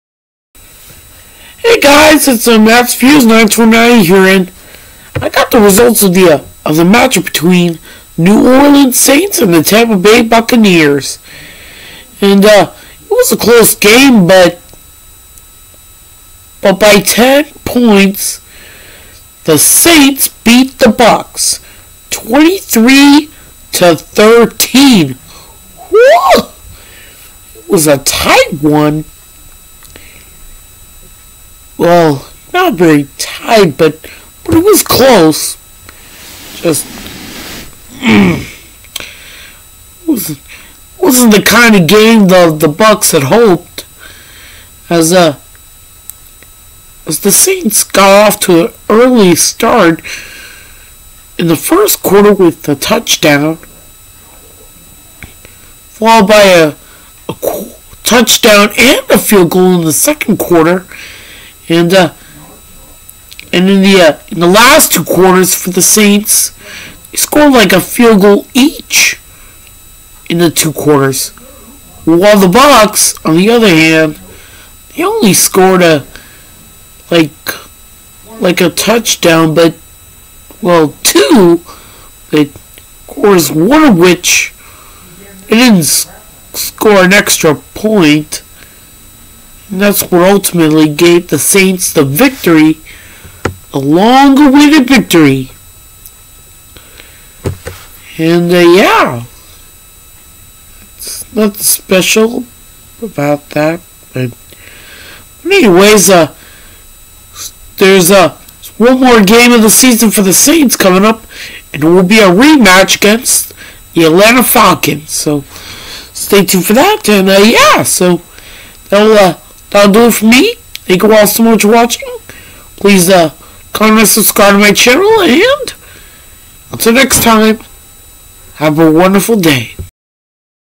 Matthew Talks About : New Orleans Saints Vs. Tampa Bay Buccaneers : Recap!
I've got the results of the match-up between the Saints & the Bucs.